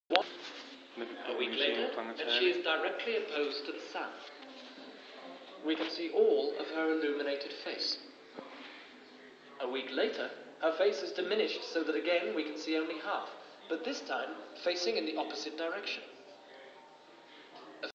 RIP Sir Patrick Moore
Spending the day at Liverpool Museum Space centre and Planetarium. Remembering the late Sir Patrick Moore RIP.
Sent using BlackBerry® from Orange